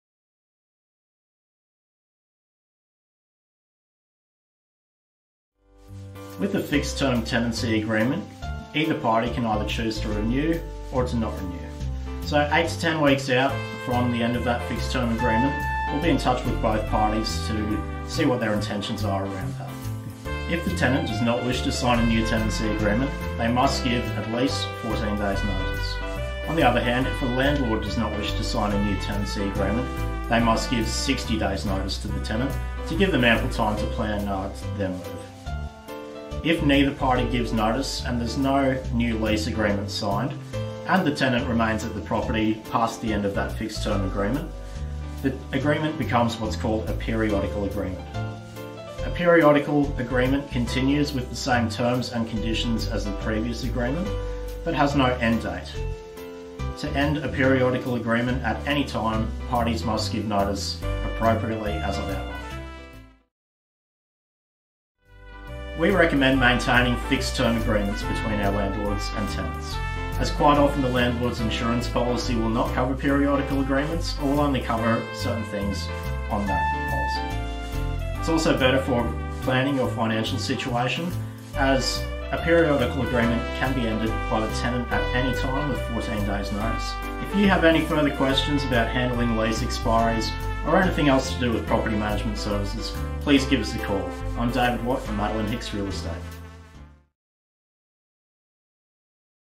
What Happens at the End of a Tenancy Lease?
What happens at the end of a tenancy lease? If you have any questions about this process please contact Madeleine Hicks Real Estate's property management team on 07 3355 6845 - we're happy to help.